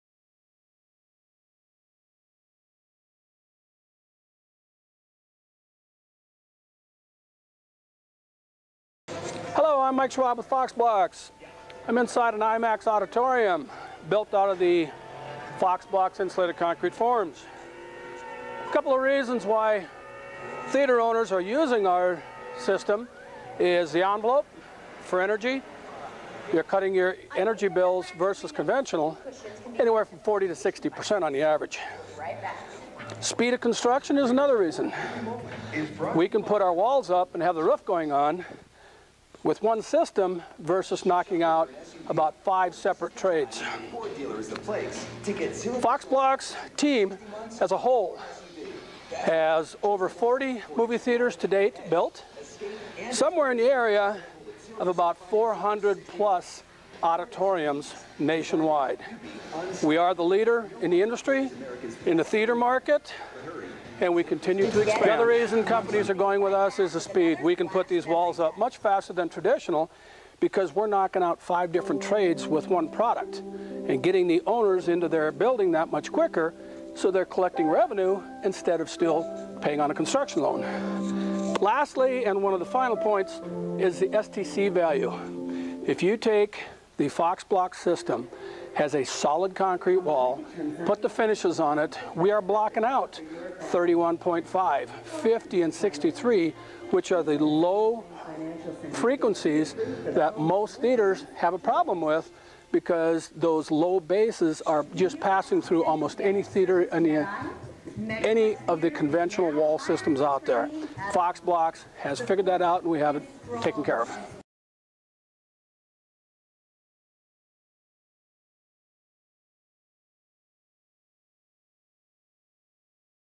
Fox Blocks and Why Theatres are Specifying ICF
Theatre owners and architects are demanding faster builds and higher quality sound experience for their customers. ICF's deliver both. Plus a unique lower operational cost due to heating cooling load reductions.